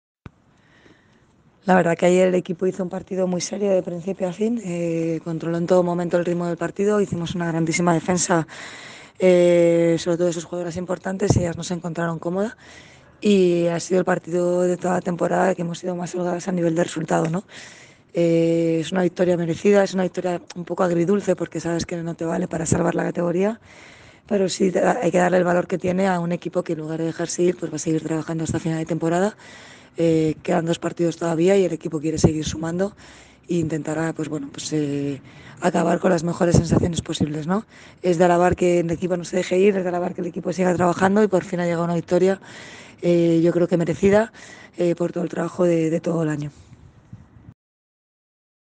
🗣🎙 Declaraciones de Bea Pacheco (Entrenadora CB Maresme Boet 3V Femeni) tras la jornada 28 🏀 Liga
⬜🟧 Declaraciones de Bea Pacheco (Entrenadora CB Maresme Boet 3V Femeni) tras la jornada 28 🏀